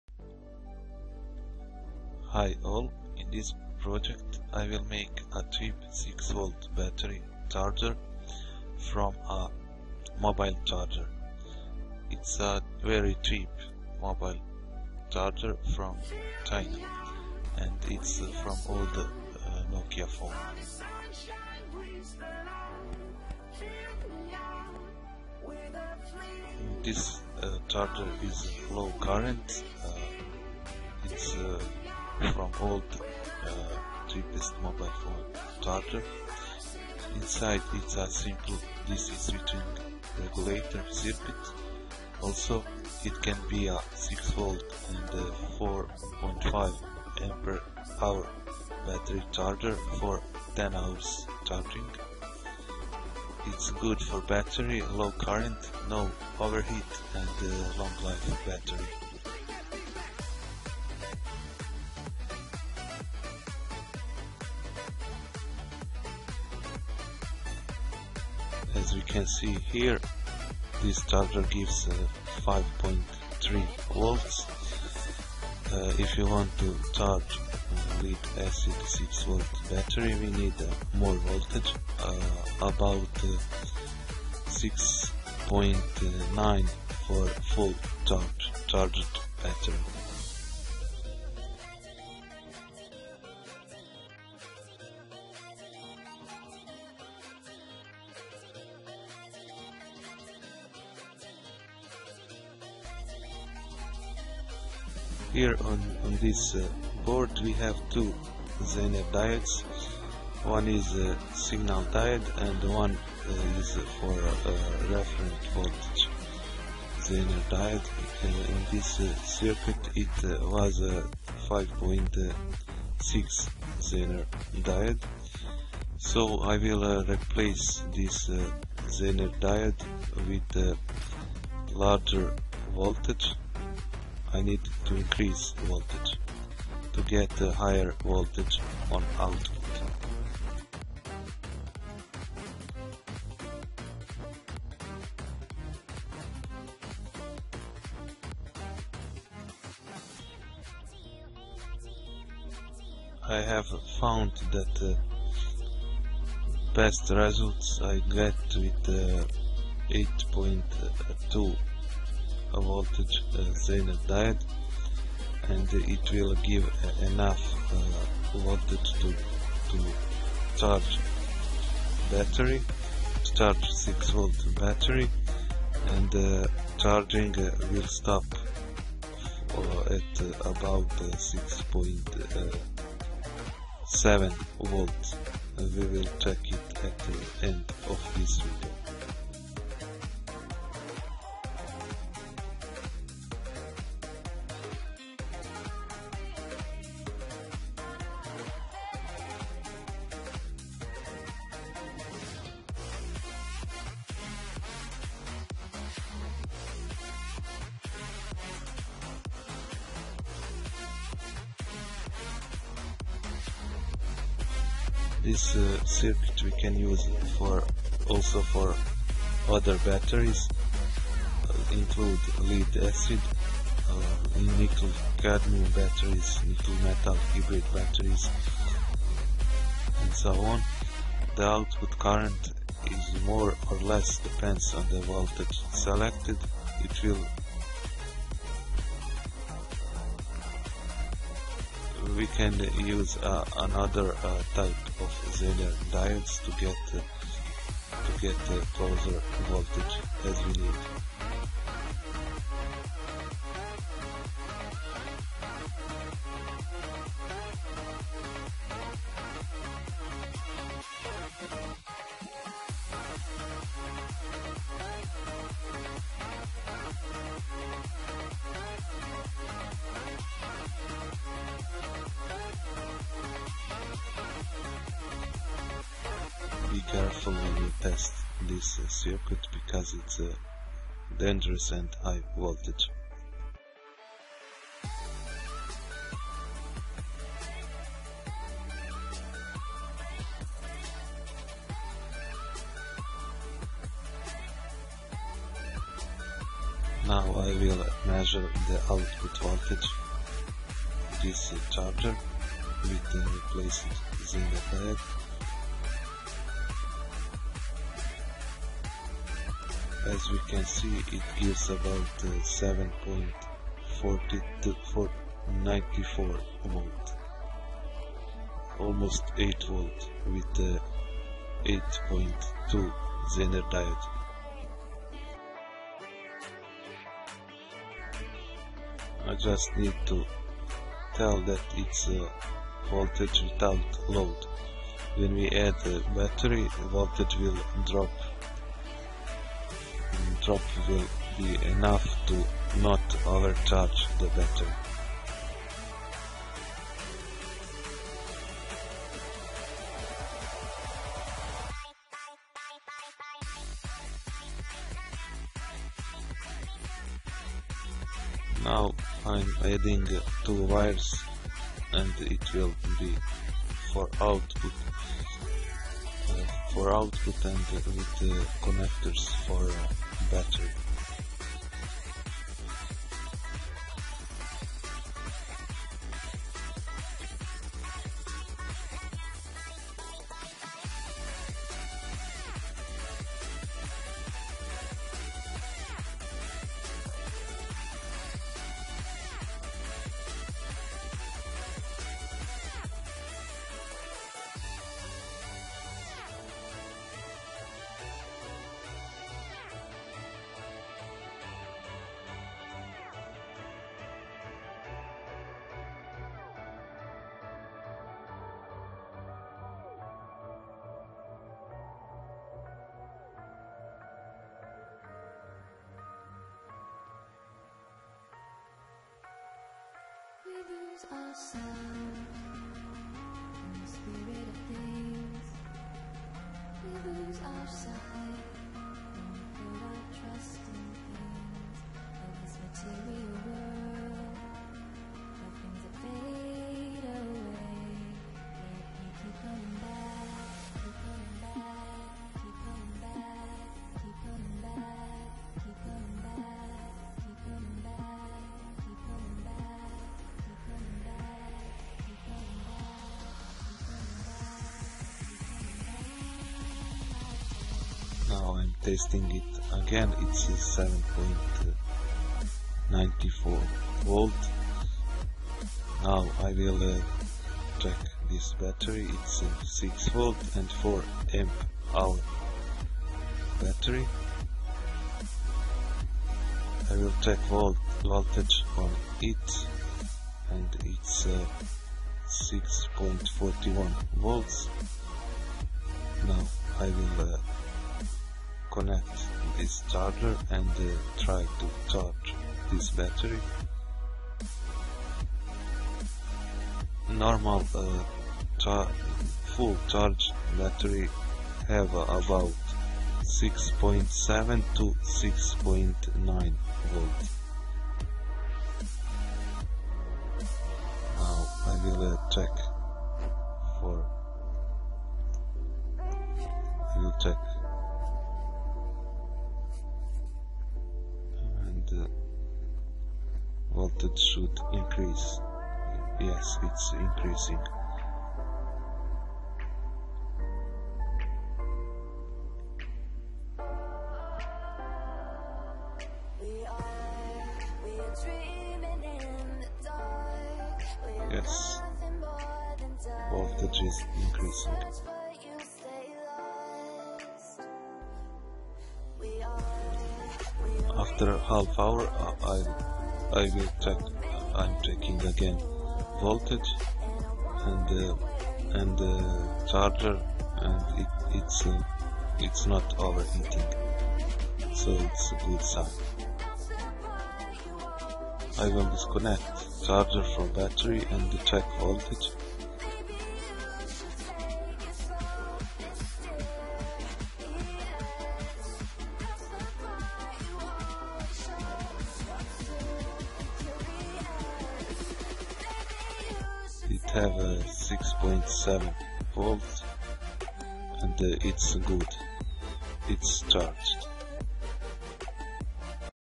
Make cheap 6V battery charger circuit from a mobile charger lead acid, ni-mh, ni-cd
Probably most of us have some old phone charger that gives 5V output.
I have one device that use 6V lead acid battery, but original charger burned and I didn't have other solution only make one.
Inside it is a simple DC switching regulator circuit. Also, it can be a 6V 4.5Ah battery charger for 10hr charging.
For example, a power supply for general load and a low current battery charger circuit.

This circuit we can use for charge a 6 volts lead-acid batteries.

And also useful for others: nickel-cadmium batteries,nickel-metal hydride battery(also known as NiMH), etc.

Caution:
1. For the device has various polarity includes the Electrolytic capacitors,Zener diode,all diode,transistor,LED we should be careful in the assembly circuit.
2. If the output voltage is at 12V you need change capacito at output rail to 470uf 25V.